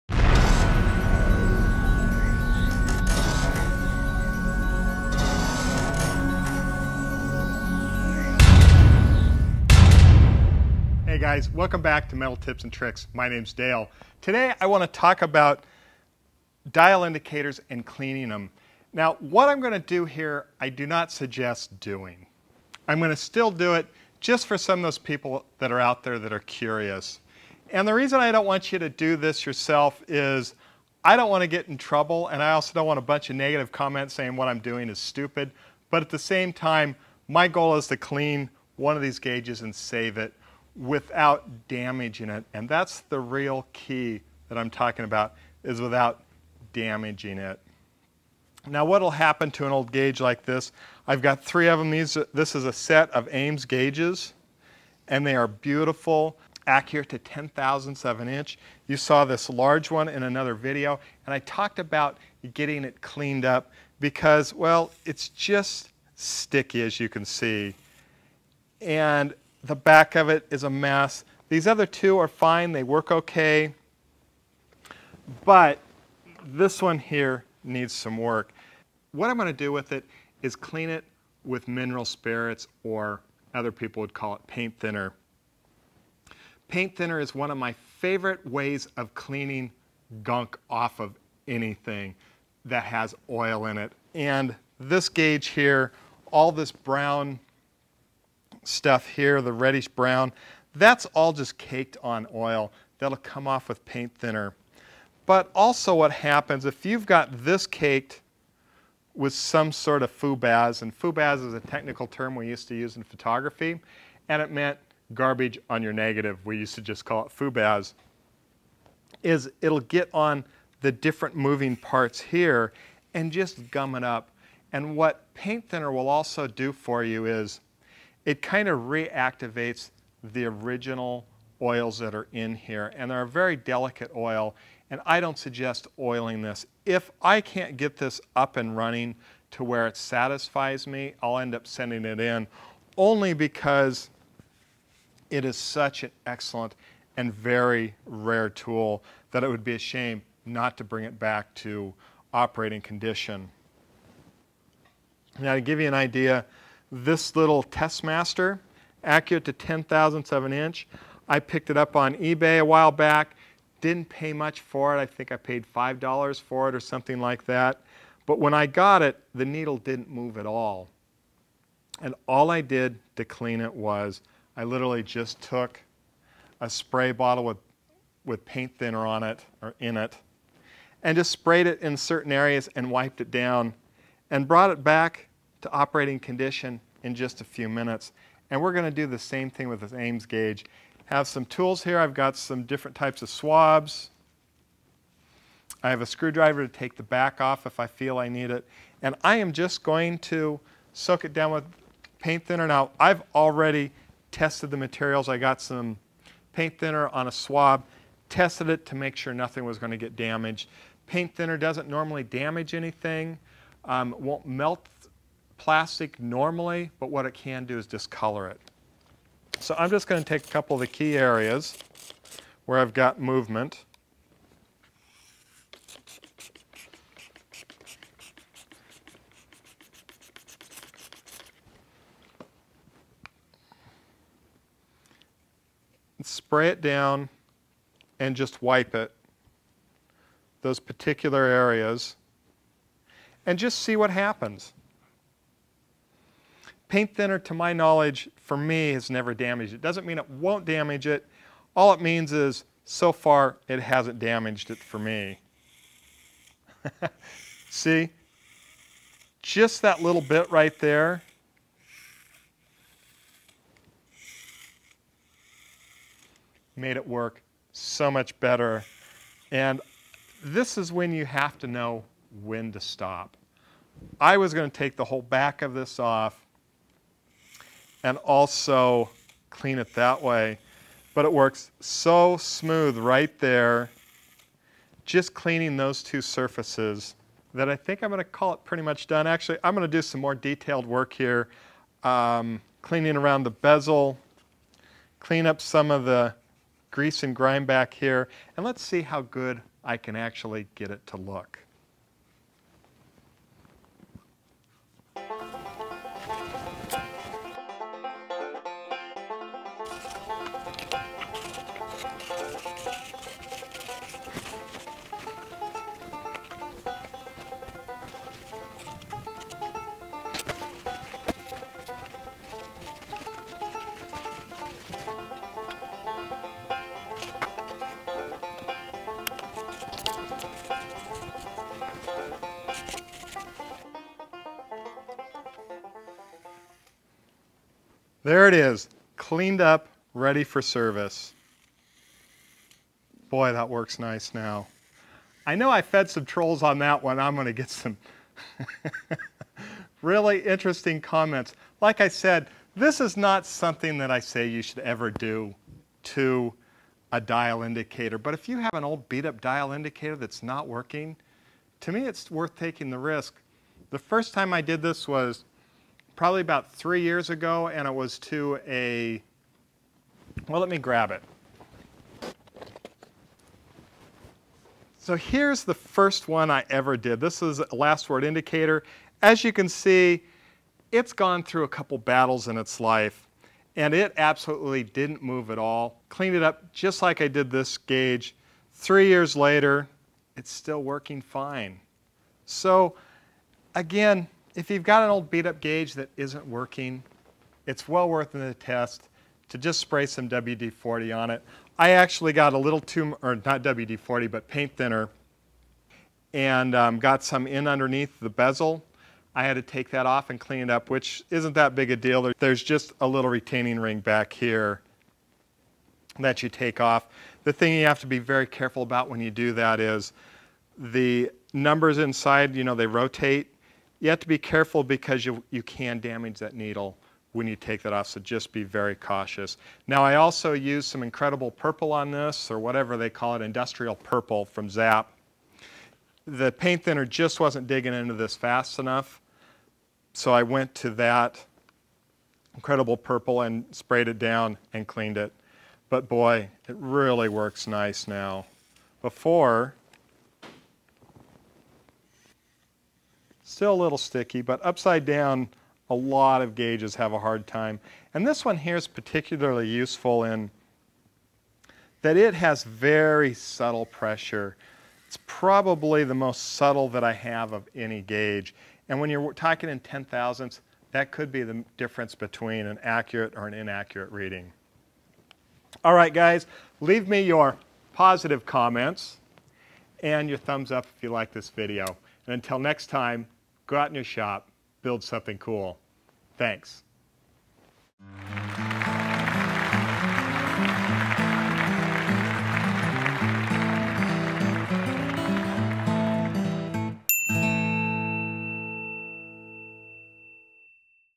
How to Clean a dial indicator
Quick and easy way to fix a Dia indicator. When indicators get dirty and quit working what do you do? Send it in for a expensive repair bill, or Watch this video and do it yourself/